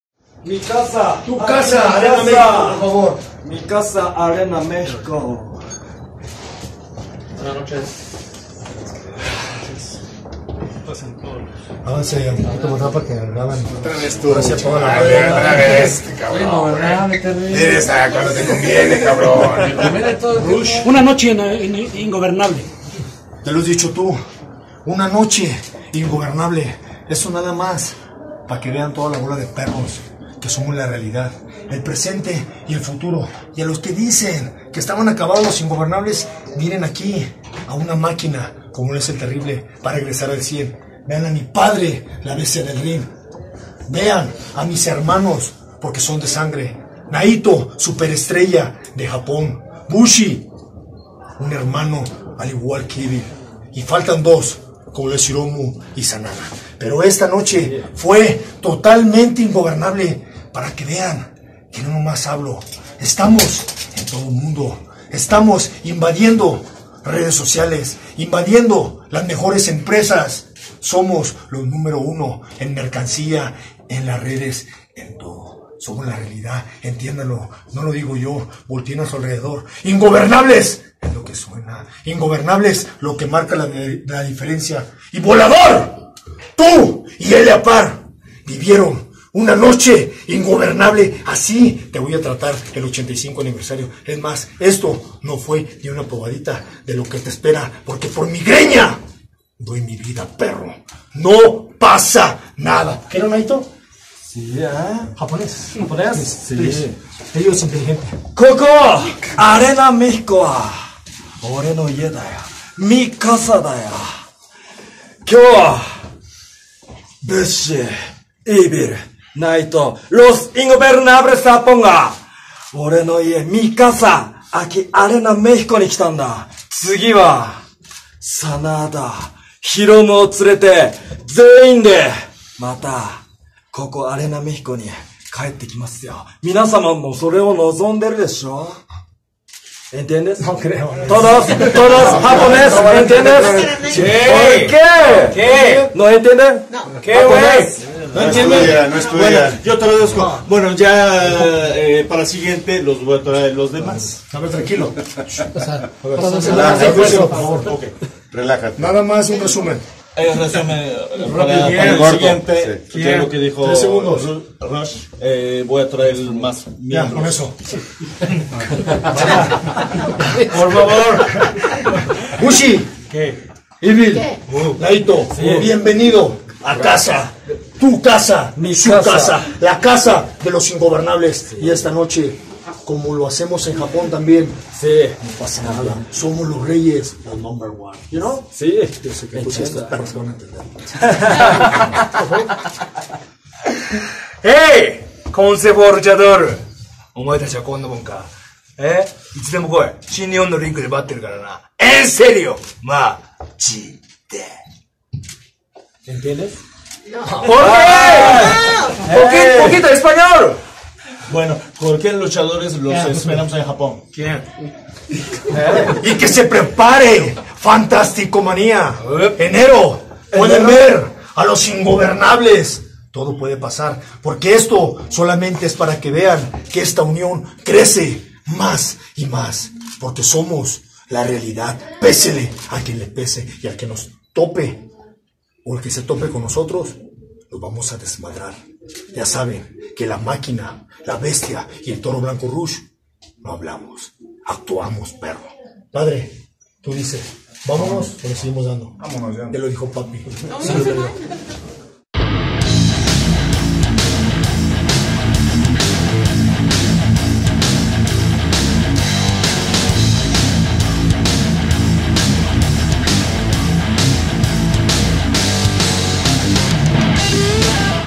“Nos faltan dos, pero esta noche fuimos totalmente Ingobernables”: Rush
En enero Los Ingobernables en FantasticaManía